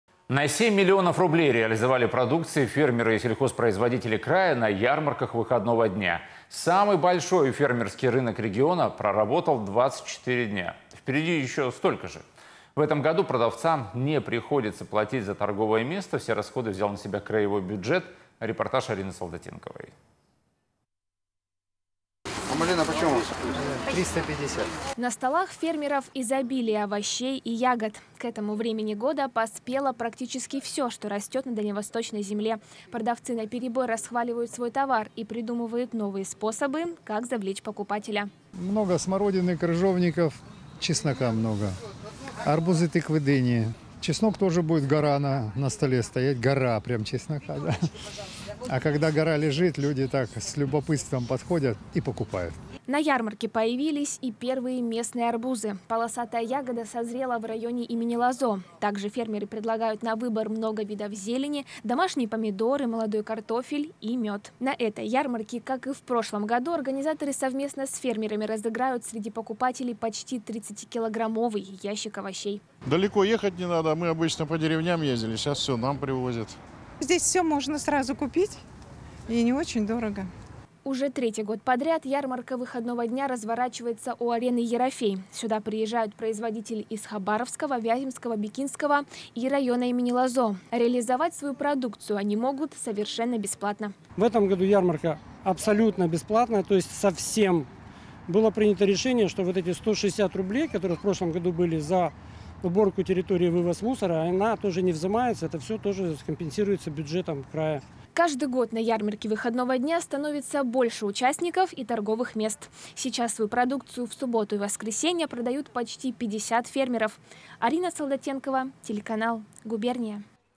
На ярмарках выходного дня хабаровчане наторговали на семь миллионов рублей. Новости. 27/07/2022
На 7 миллионов рублей реализовали продукции фермеры и сельхозпроизводители края на ярмарках выходного дня. Самый большой фермерский рынок региона проработал 24 дня. Впереди ещё столько же. В этом году продавцам не приходится платить за торговое место, все расходы взял на себя краевой бюджет.